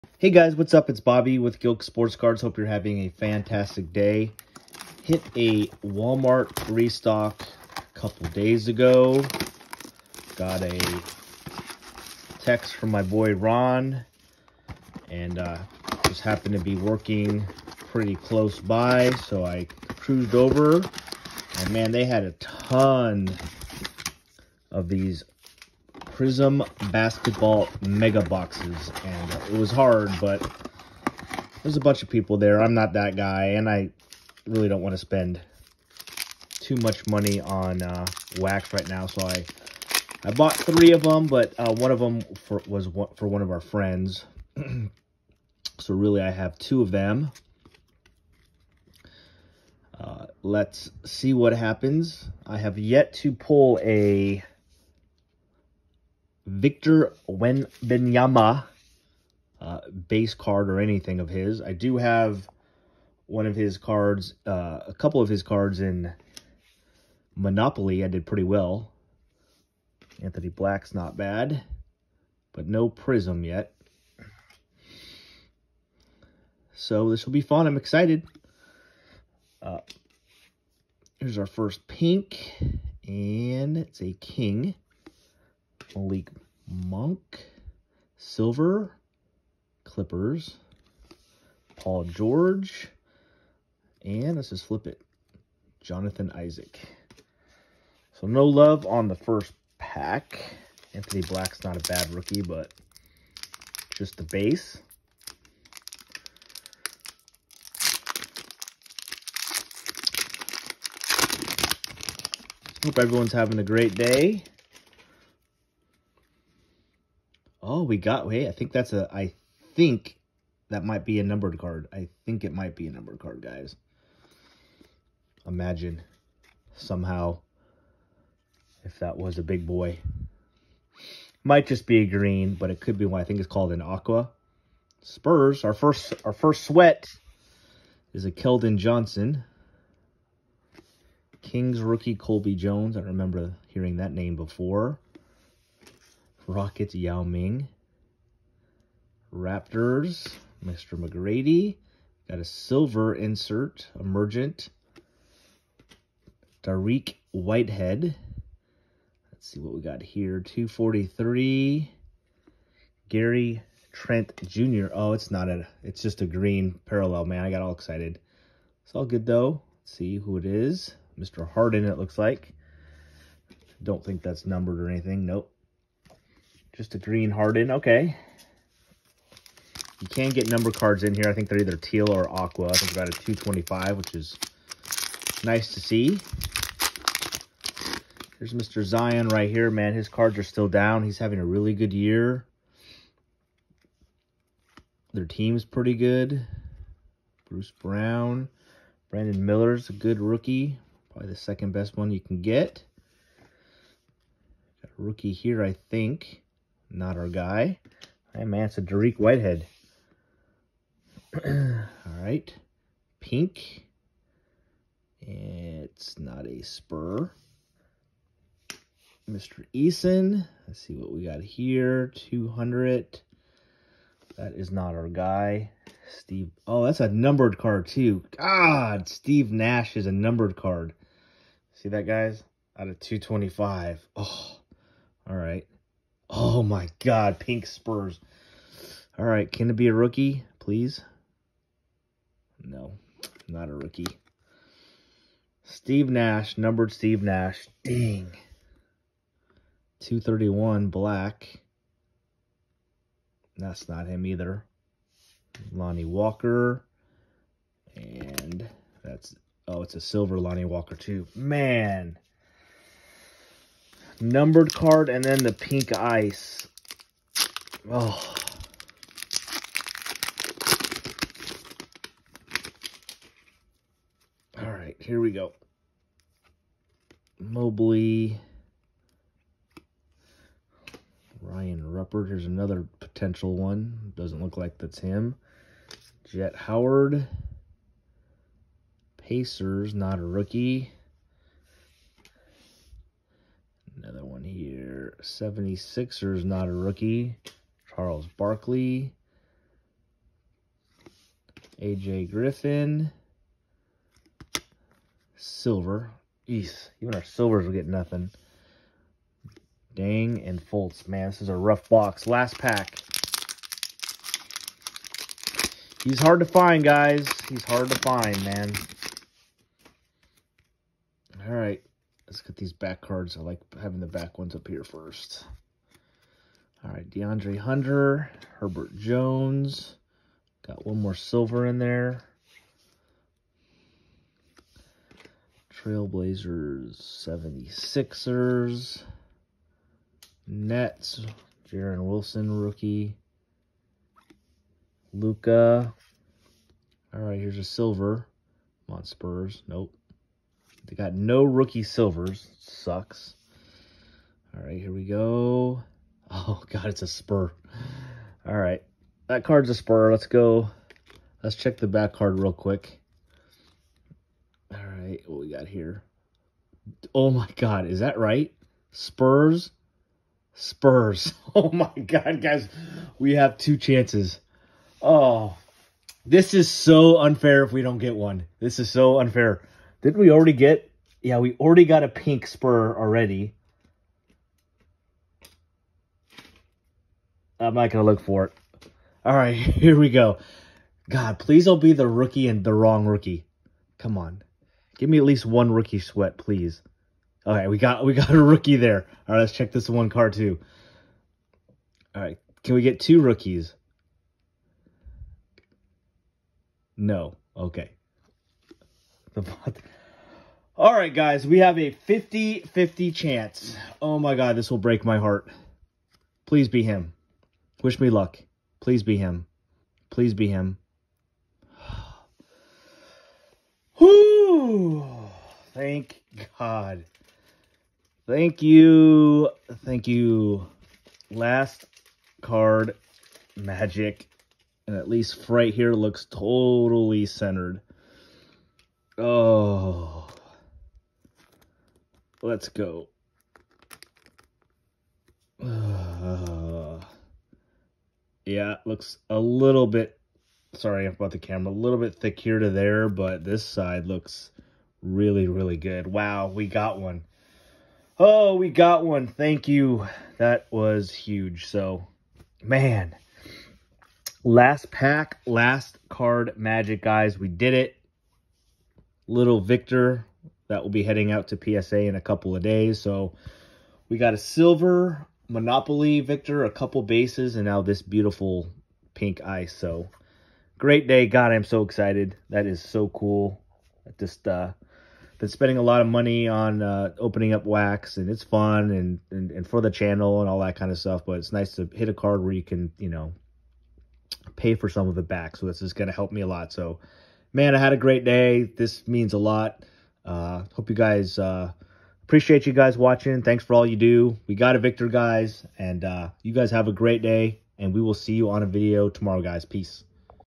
Massive Pull 👀- 2023 Prizm Basketball Mega Box - We did it 🤣🎉!!
Took a shot at 2023 Prizm Basketball and hit a bomb in a mega box.  Last card Magic baby !